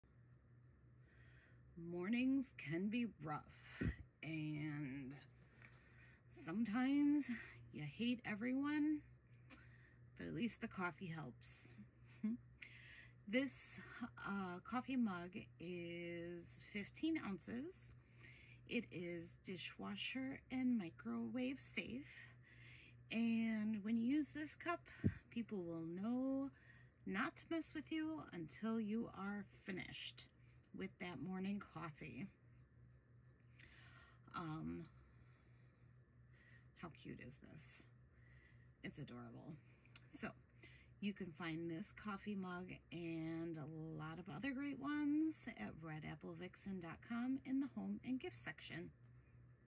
Skeleton Coffee Mug
You know how mornings can be. You wake up and find out the world is still waiting for you, just when you wanted to go back to sleep. Coffee helps.

This is a bright and beautiful picture of a skeleton holding their morning cup of coffee. Coffee mug is 15 ounces, dishwasher and microwave safe